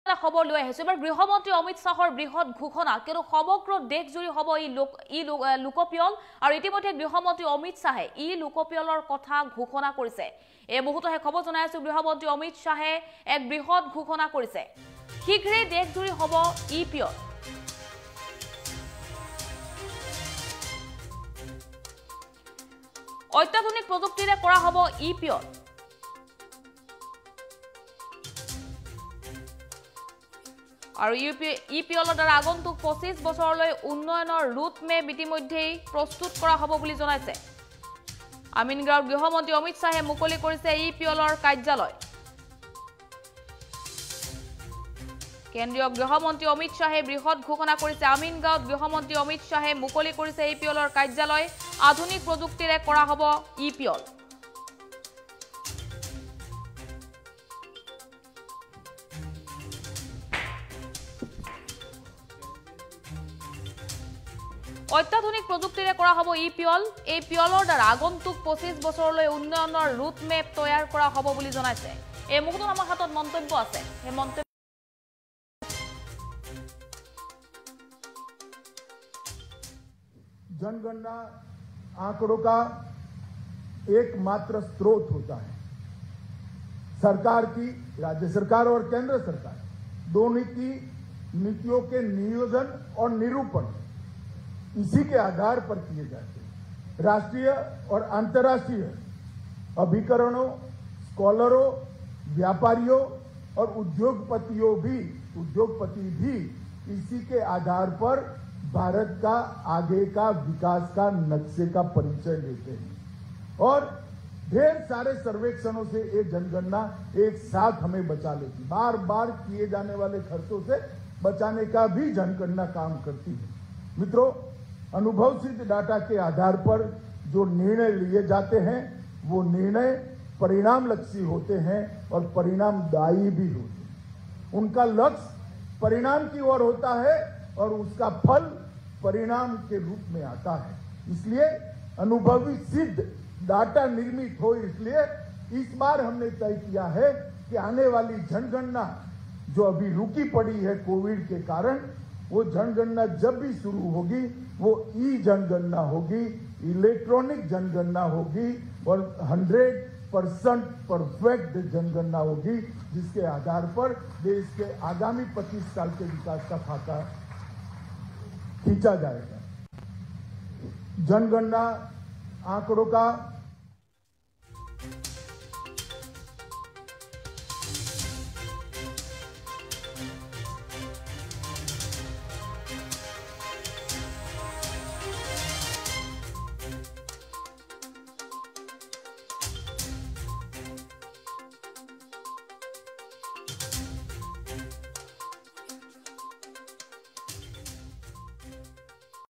কেন্দ্ৰীয় গৃহমন্ত্ৰী অমিত শ্বাহৰ বৃহৎ ঘোষণা
শীঘ্ৰে দেশজুৰি হ'ব ই-পিয়ল